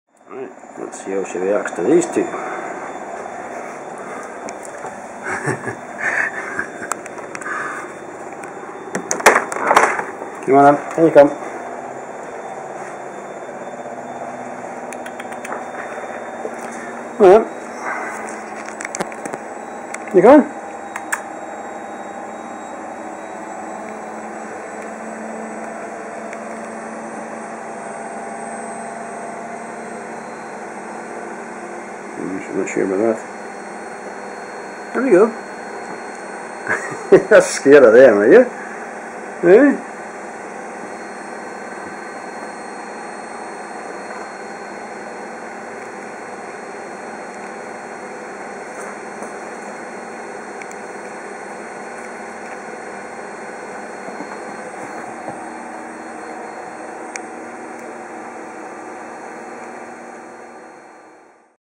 HT Meets The Gang
Grey Squirrel comes face to face with a Hedgehog and Red Squirrel for the first time...